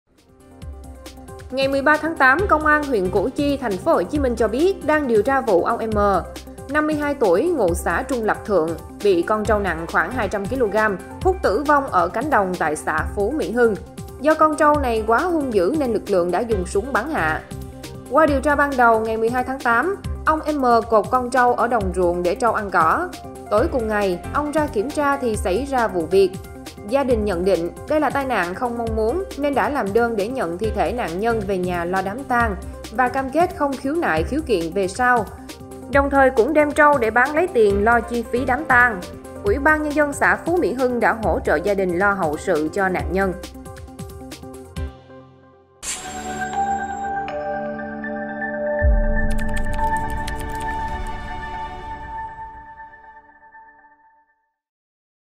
Trâu nặng 200kg húc c.h.ế.t người ở huyện Củ Chi
Ngày 13-8, Công an huyện Củ Chi (TPHCM) cho biết, đang điều tra vụ ông M. (52 tuổi, ngụ xã Trung Lập Thượng) bị trâu (nặng khoảng 200kg) húc t.ử v.o.n.g ở cánh đồng tại xã Phú Mỹ Hưng.